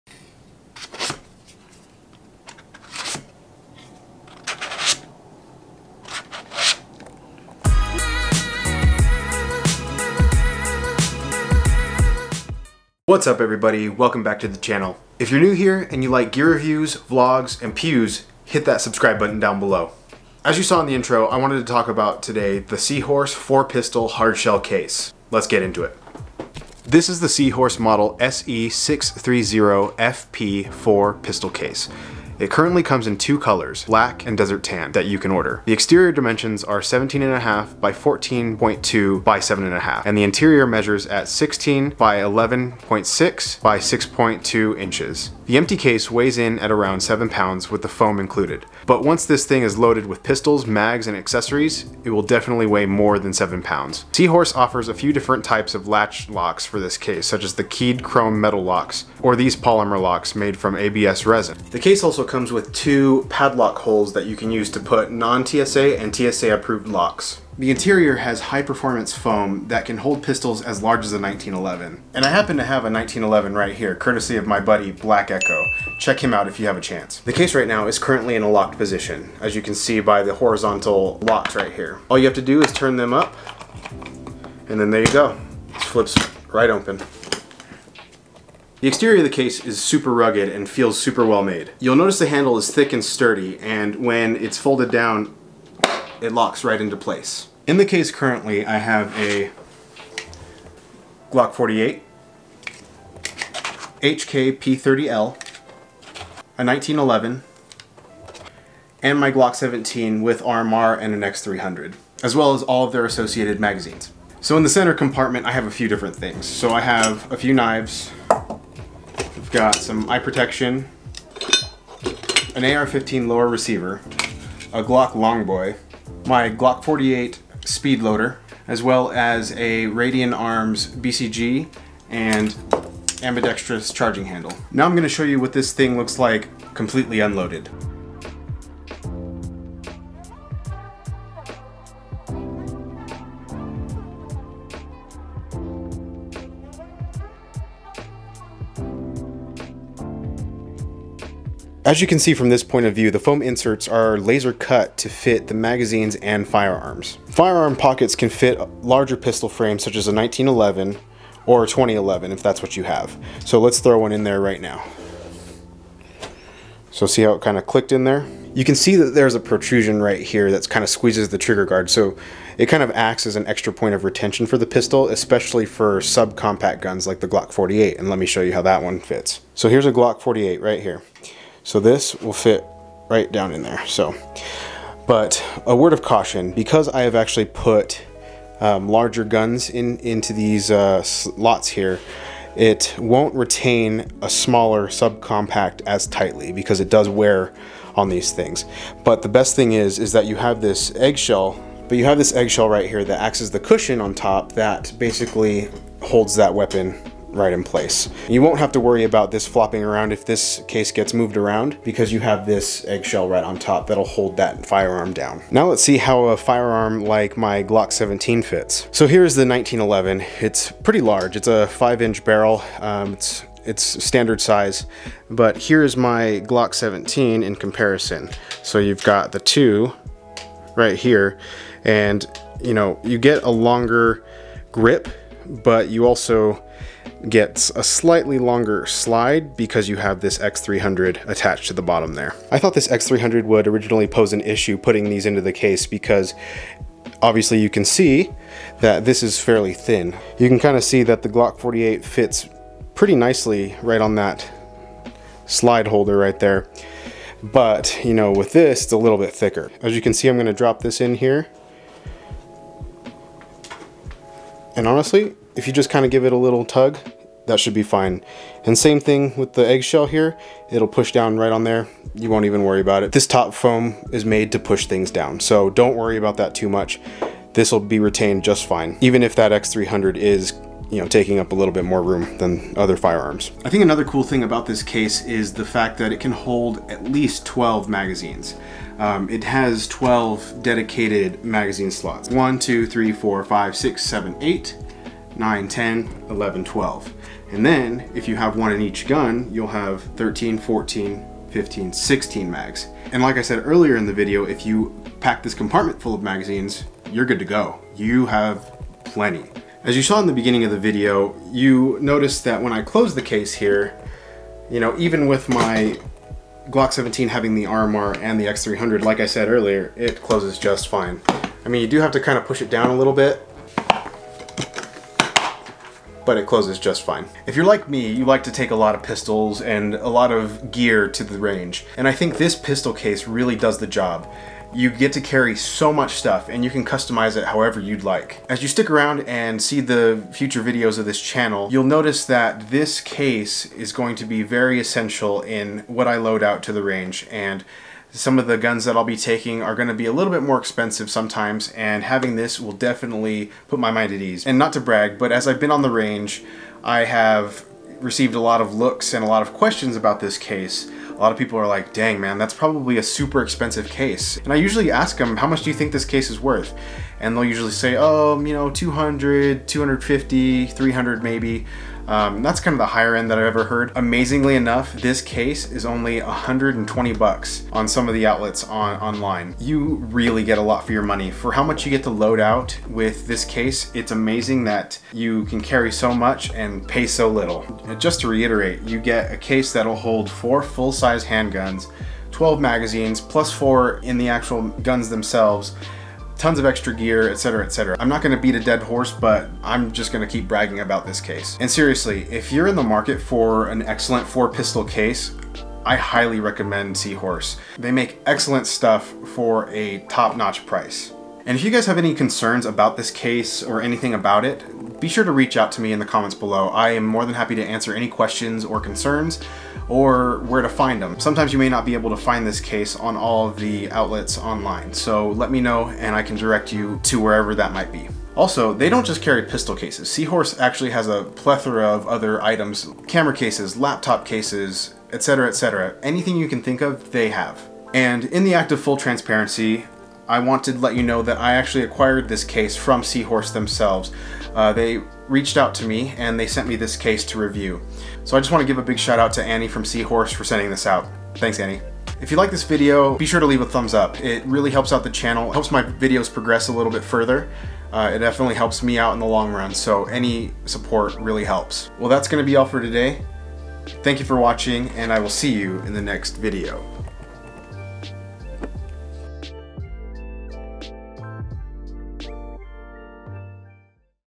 Seahorse 4 Handgun Hardshell Carrying Case! | Best Pistol Case for the Money?
Back again with another review! Today I am talking about the Seahorse 4 Pistol Case! This thing is amazing and carry carry tons of rangeday gear, mags, guns, etc. You will love the protection this case provides!

Let me know down in the comments what you use to carry your firearms to the range!

Camera I use
Lens I use
Lens Adaptor
Vlogging Mic
Light I use
Best vlogging tripod
Tripod I use
SD Card I like